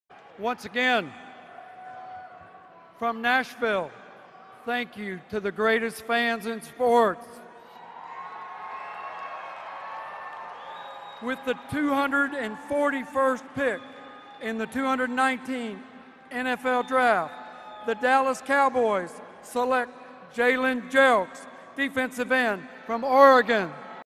Jalen Jelks Is Announced As The 241st Pick In The 2019 NFL Draft | Dallas Cowboys 2019
With the 241st Pick in the 2019 NFL Draft, the Dallas Cowboys select, Jalen Jelks from the University of Oregon.

Tune-in for all @NFLDraft coverage on ABC, ESPN, NFL Network & online at NFL.com/Watch